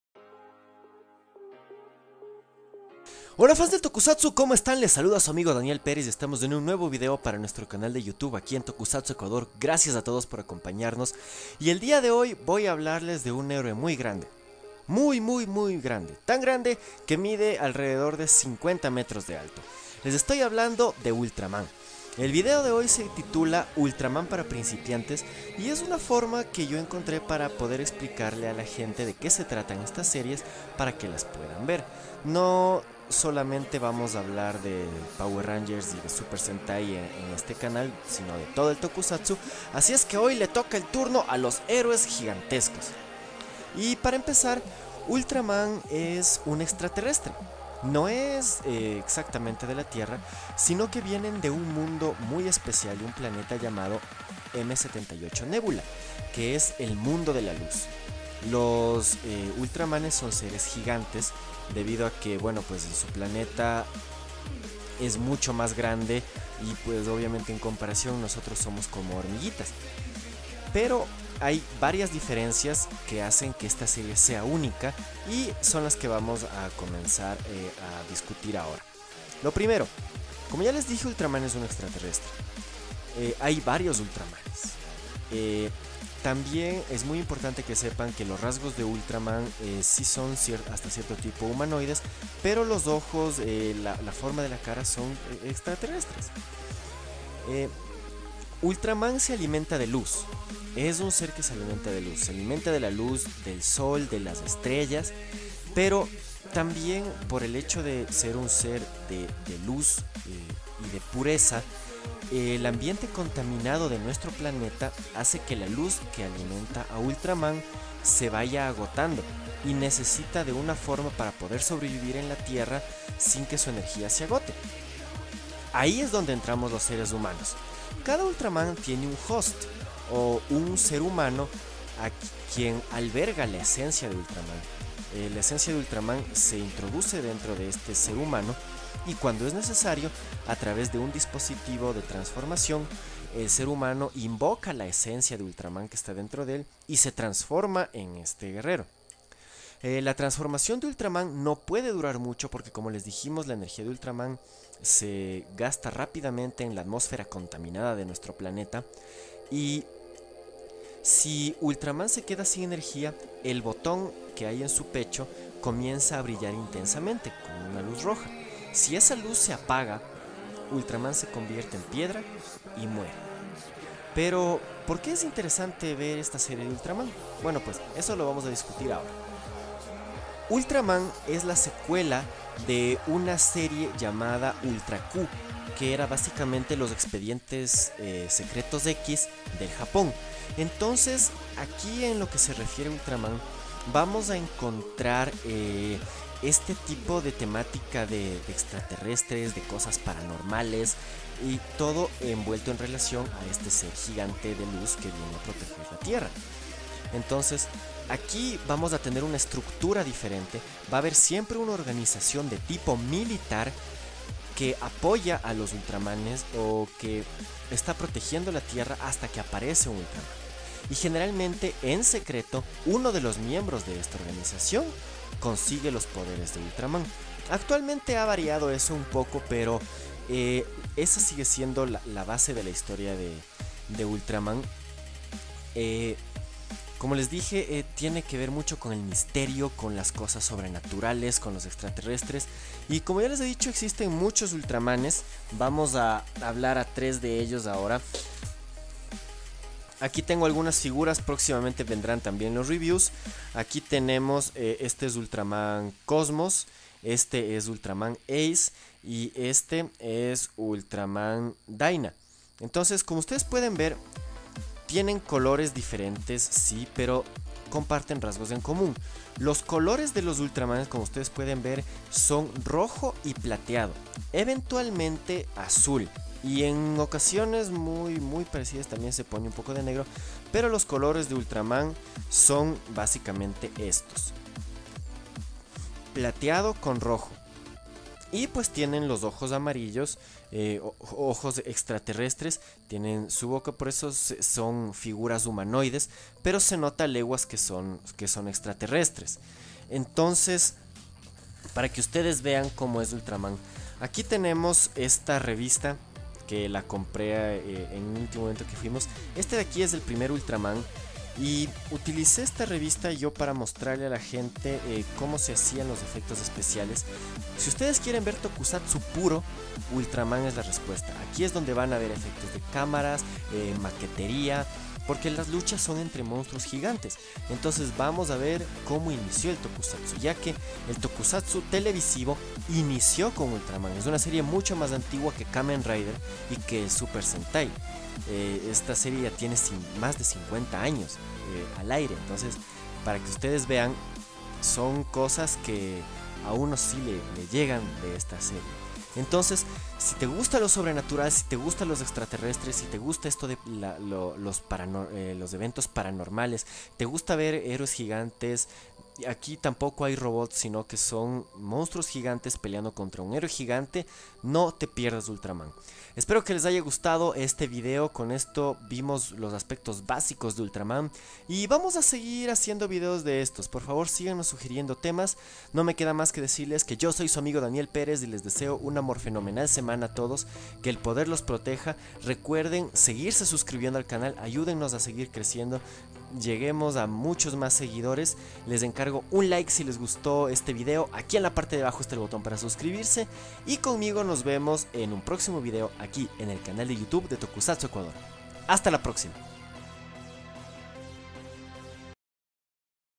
Ultraman para principiantes
Un video para quienes quieren conocer a Ultraman, no se lo pierdan !